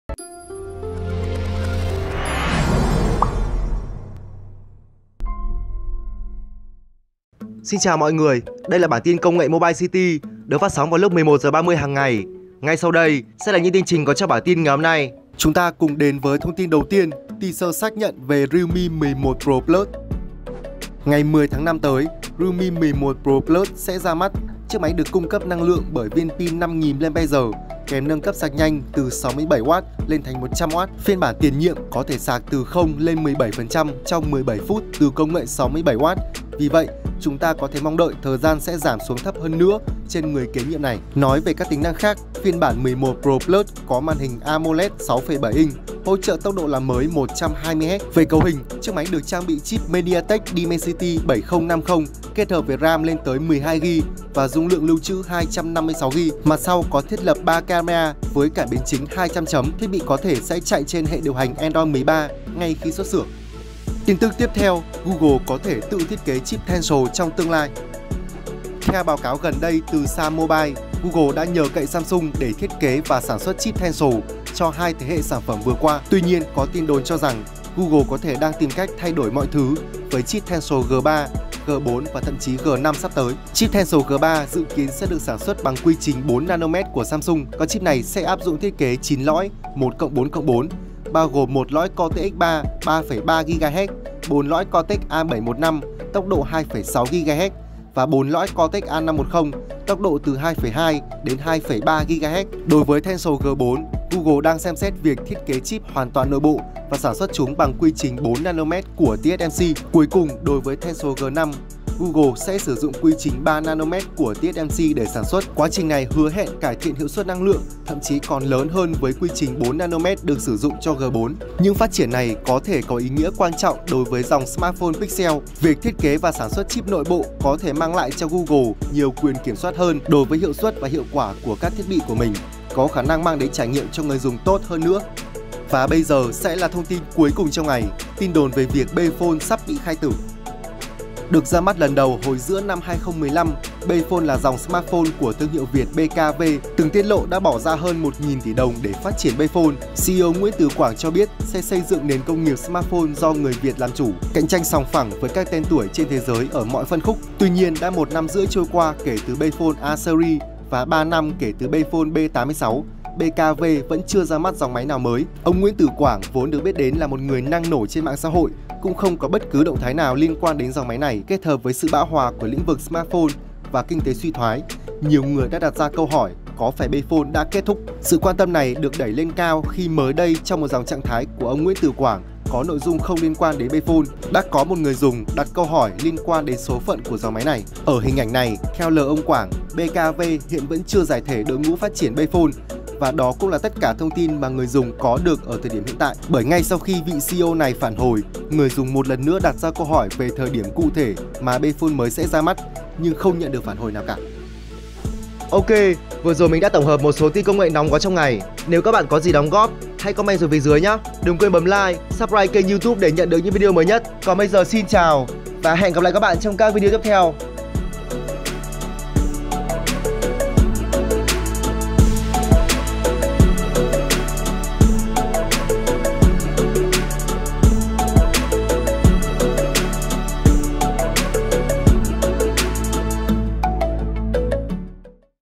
Tin nóng: Bphone sắp bị khai tử chăng?? Google tự sản xuất chip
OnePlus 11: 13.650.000

Nội dung:
0:00 Intro
0:18 Teaser xác nhận về Realme 11 Pro+
1:04 Google có thể tự thiết kế chip Tensor trong tương lai
2:18 Tin đồn về việc Bphone sắp bị khai tử